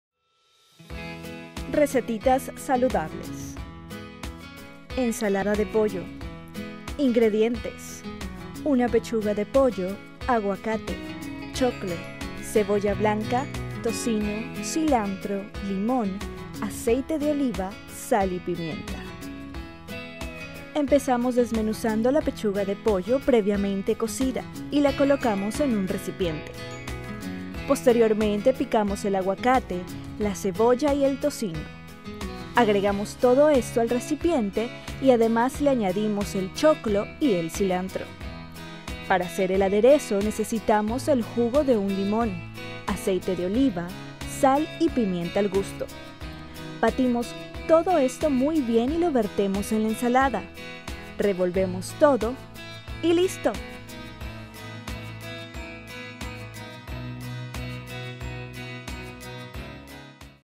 Ensalada de pollo | Recetitas
Ingredientes:
*Pollo
*Aguacate
*Choclo
*Cebolla
*Cilantro
*Limón